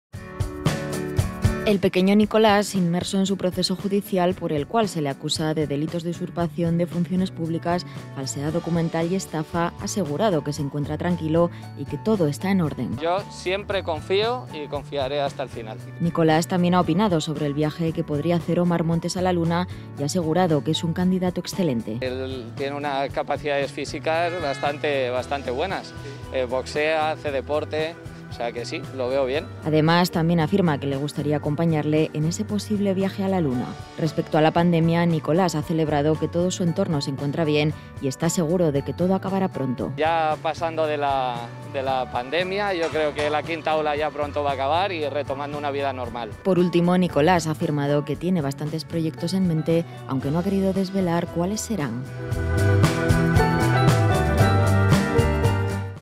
El pequeño Nicolás sigue confiando en la justicia
El pequeño Nicolás, inmerso en su proceso judicial por el cual se le acusa de delitos de usurpación de funciones públicas, falsedad documental y estafa, ha asegurado que está tranquilo y que todo está en orden.